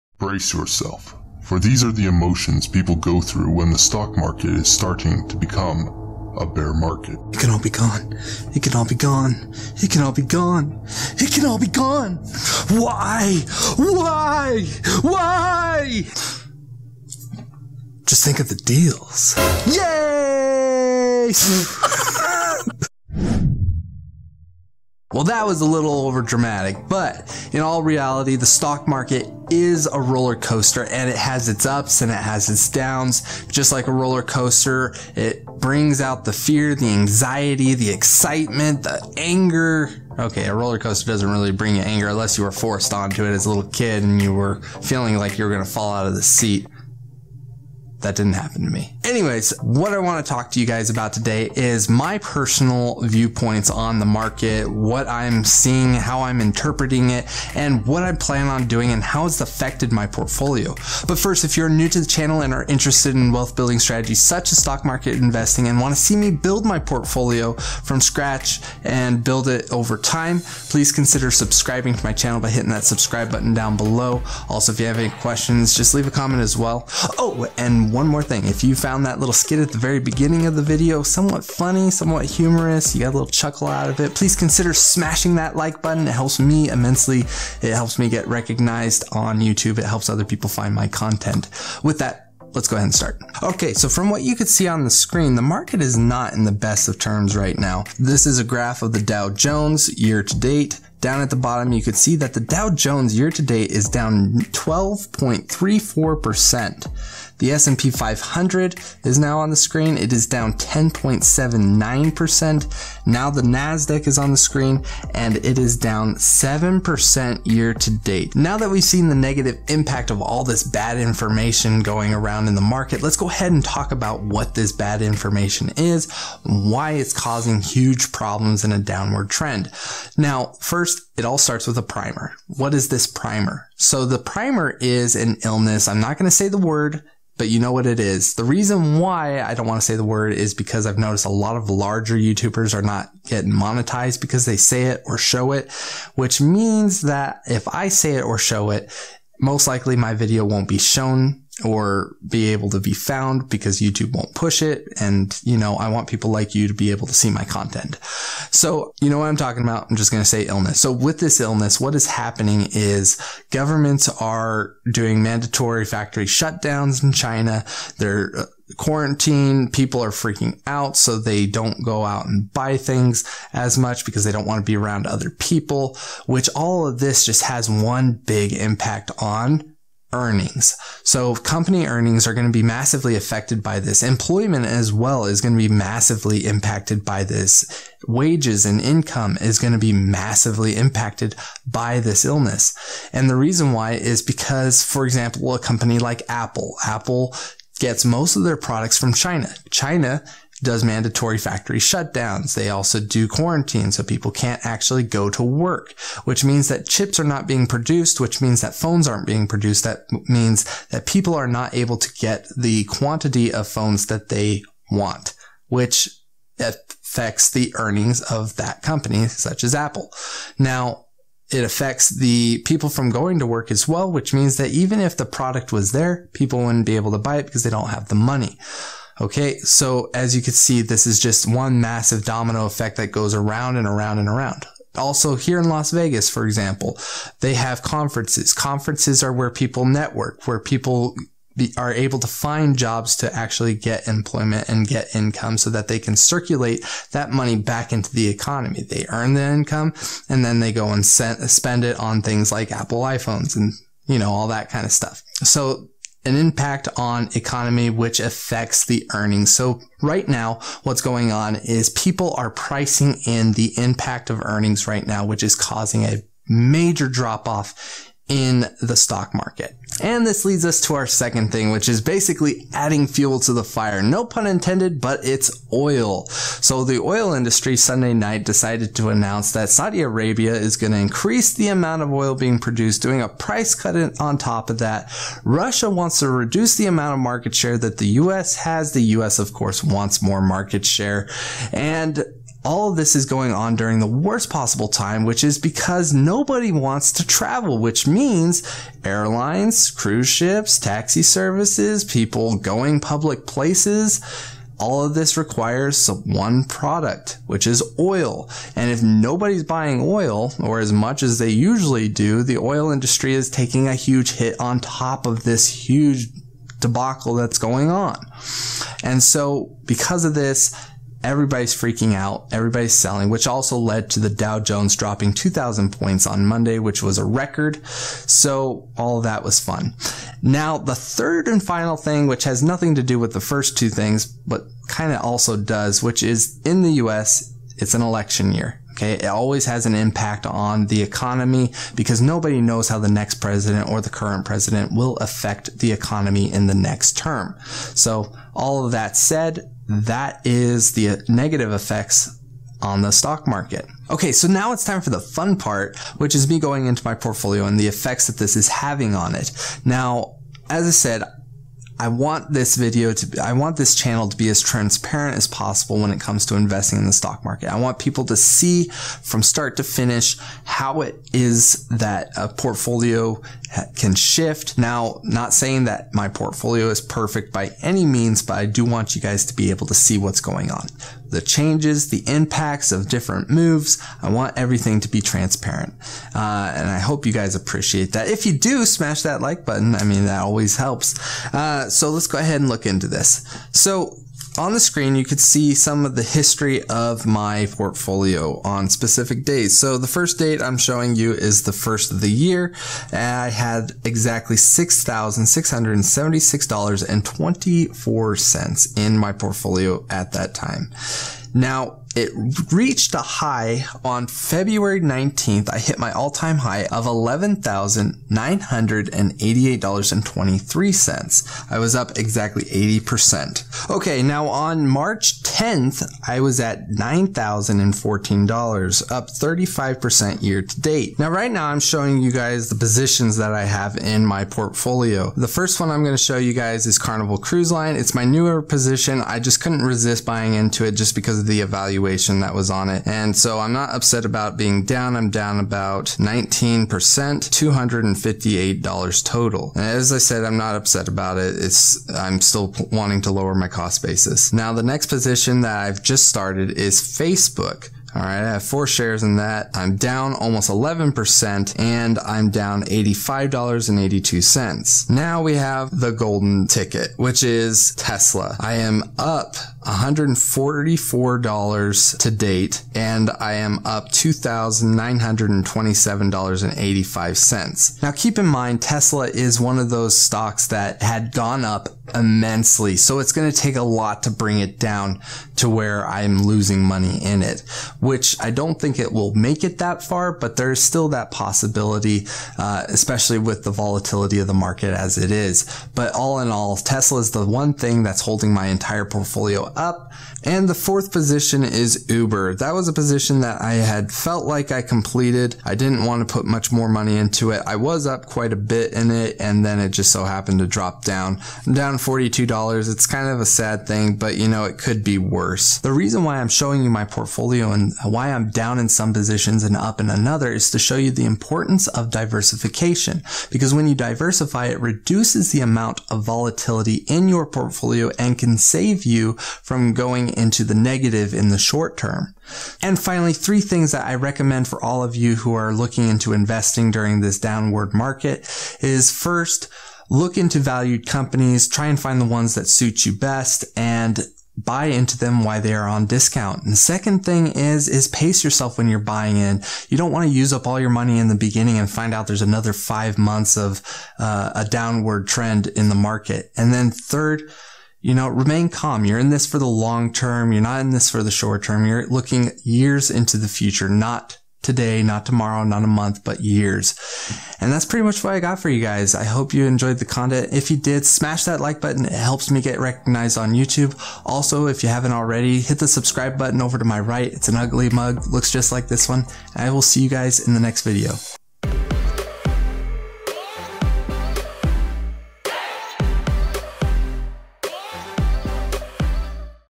Why the stock market is falling and what are my thoughts on it
Hope this helps Wealth Giants,

Ryan

Intro Credit to ONdraid from panzoid